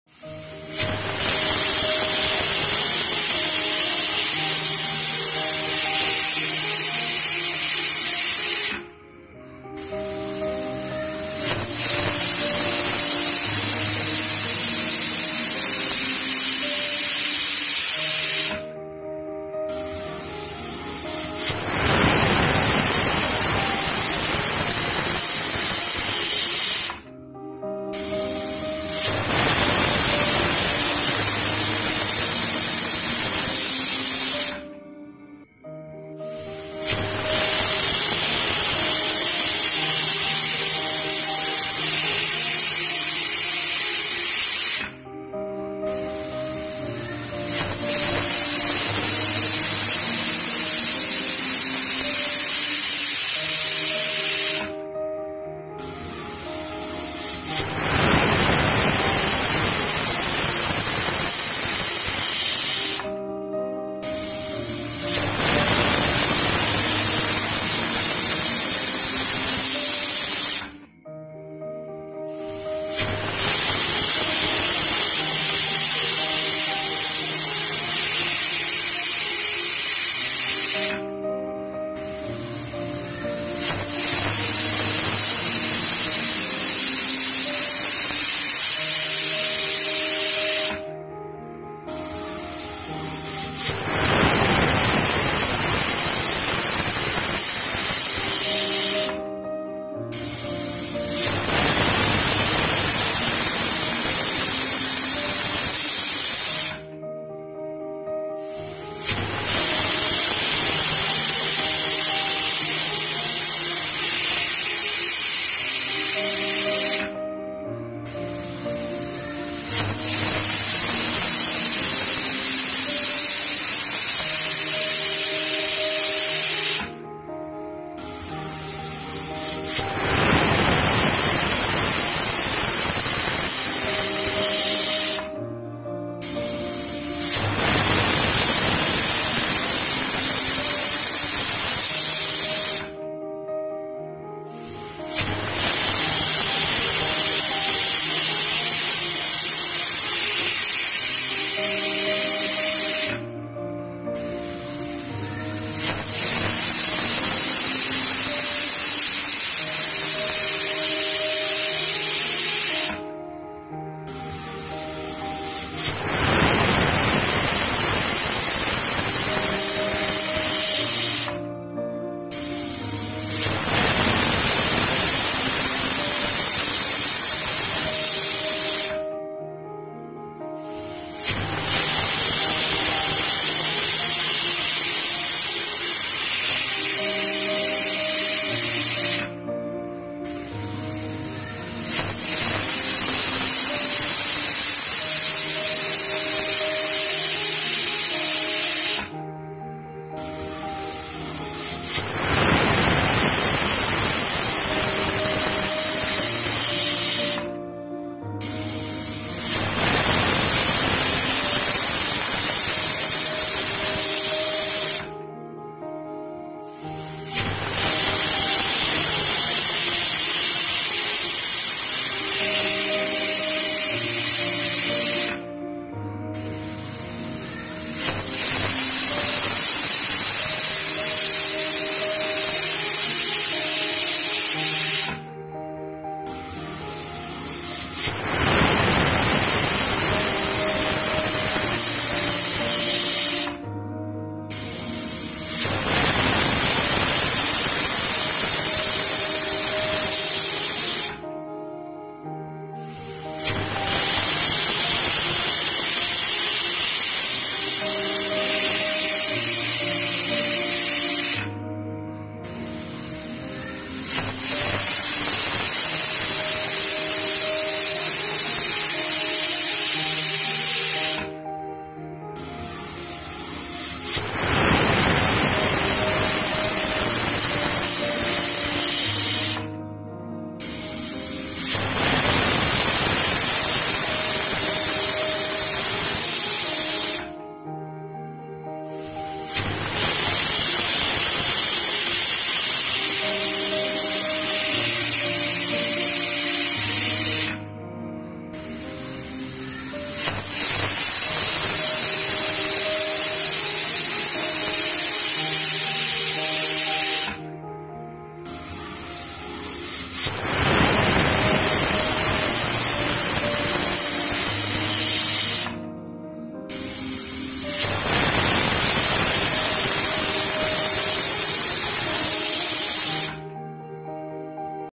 pressure cooker sound 5:24 mins |பிரஷர் குக்கர் சவுண்ட்
Pressure cooking is the process of cooking foodunder high pressure steam, employing water or a water-based cooking liquid, in a sealed vessel known as a pressure cooker. High pressure limits boiling, and permits cooking temperatures well above 100 °C (212 °F) to be reached.

Almost any food that can be cooked in steam or water-based liquids can be cooked in a pressure cooker.